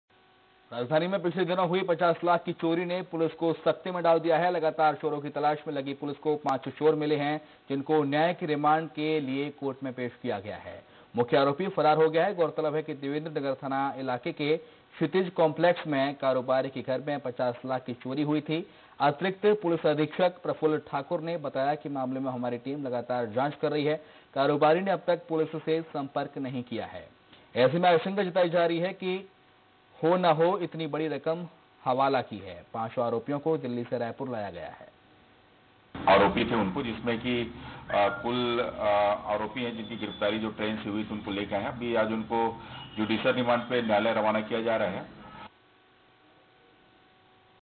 RAIPUR CRIME NEWS: 50 लाख की चोरी मामले में पांच आरोपियों को गिरफ्तार
Raipur Police Chhattisgarh Police छत्तीसगढ़ पुलिस